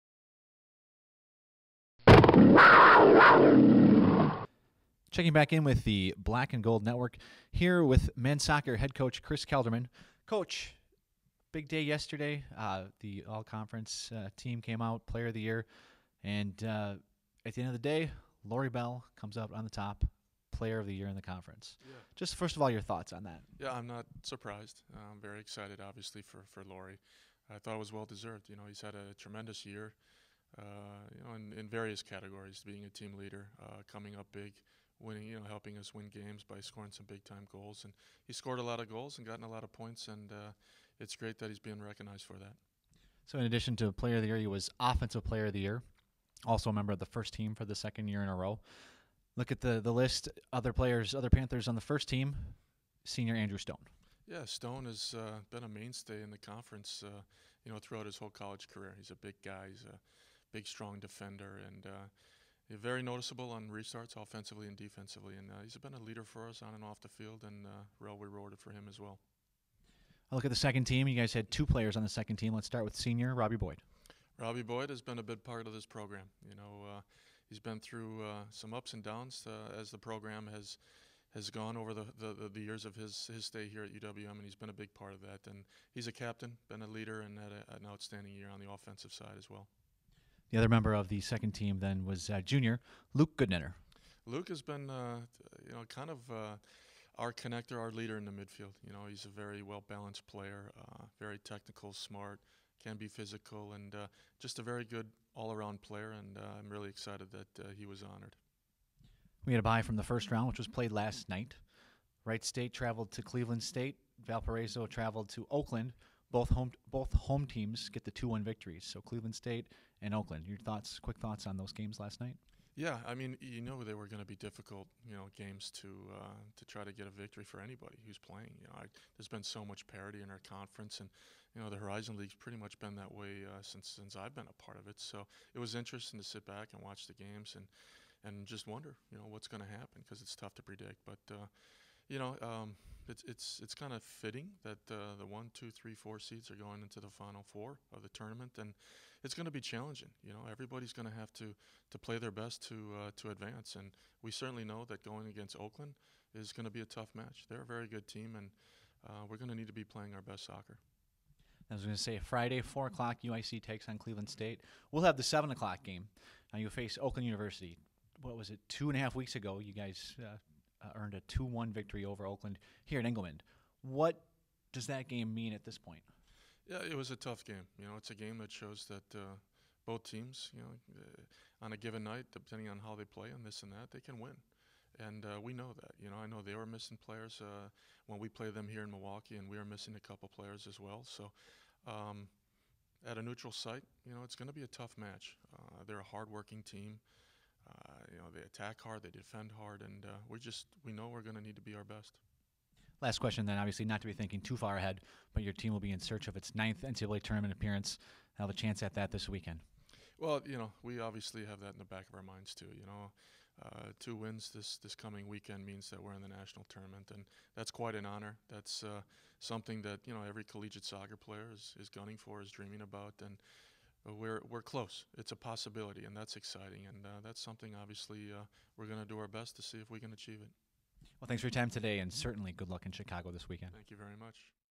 MSOC: Postseason Discussion With Coach Kelderman
With the Horizon League Tournament getting started for the men's soccer team this Friday, the Black & Gold Network chatted with head coach Kris Kelderman this week. Topics discussed included the Panthers selections to the all-conference team, the first-round games Tuesday and the expectations for UWM this weekend in Chicago.